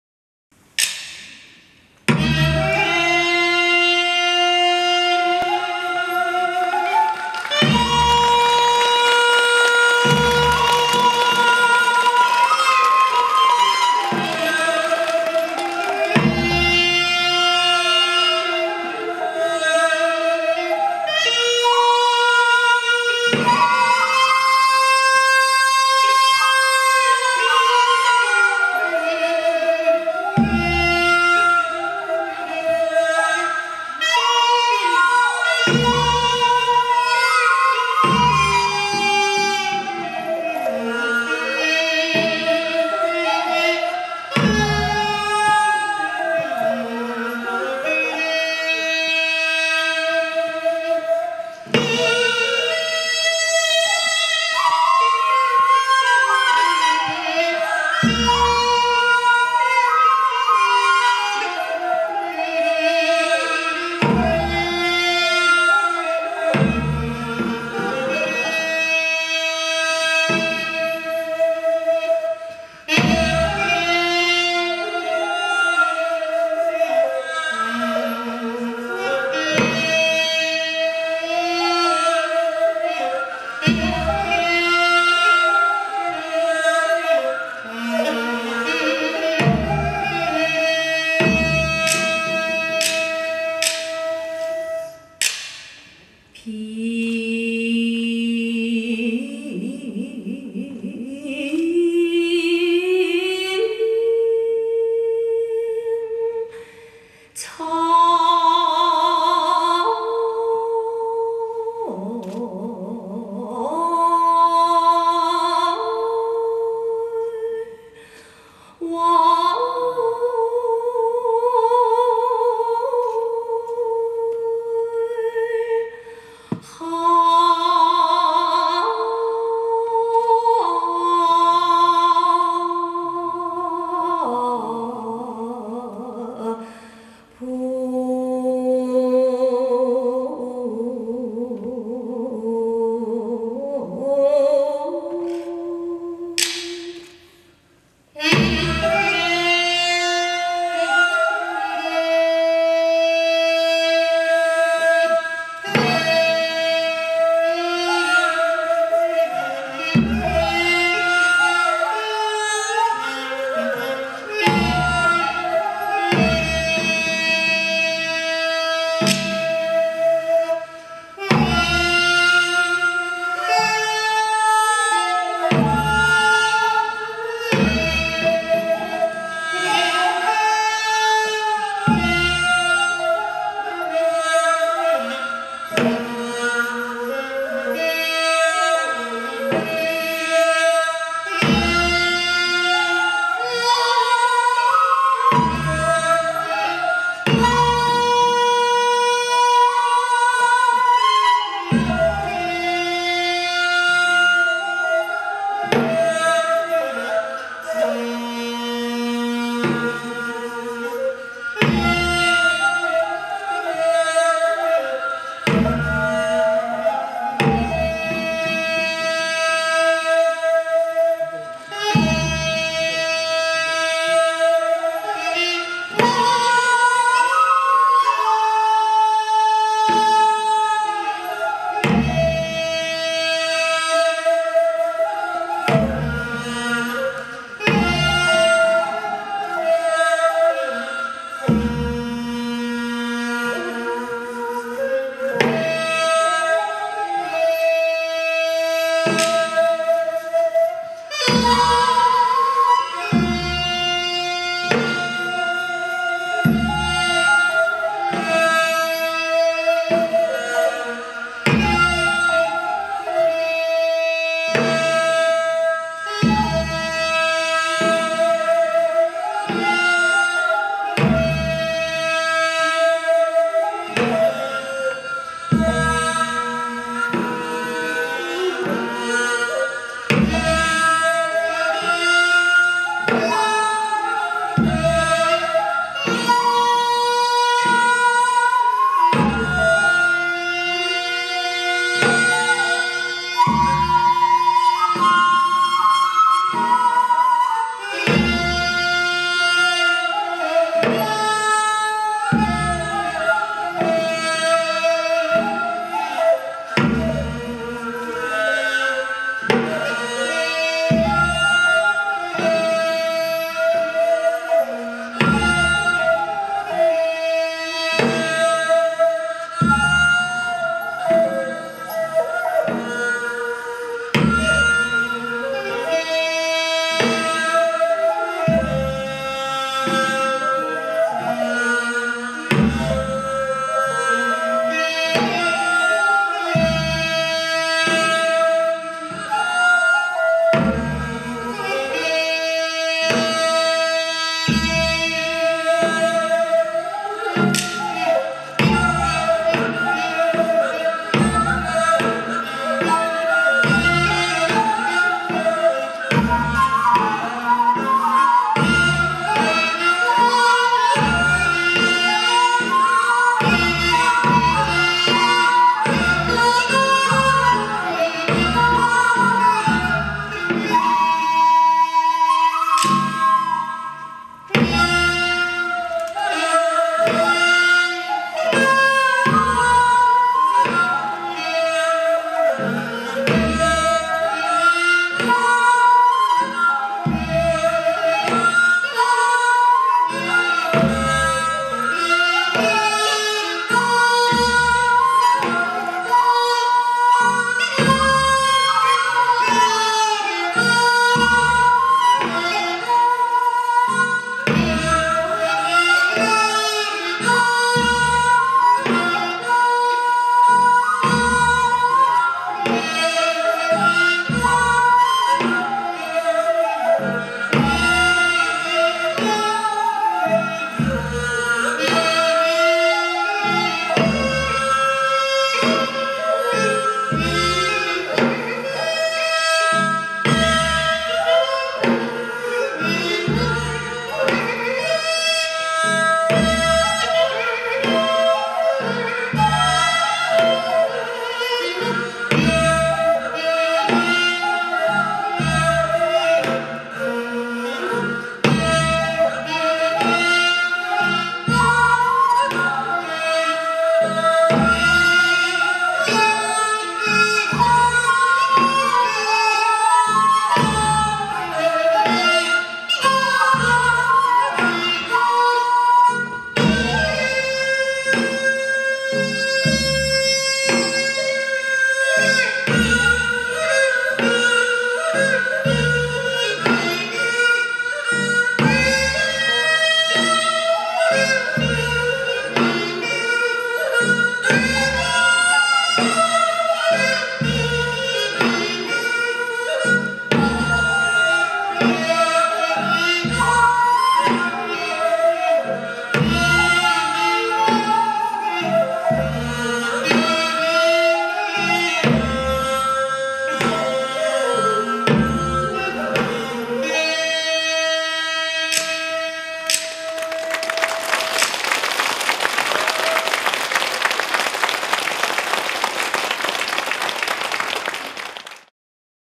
춘앵전 임수정
행사:향연(饗宴)
일시:2011년 12월 8일
장소:함양문화예술회관
출연:임수정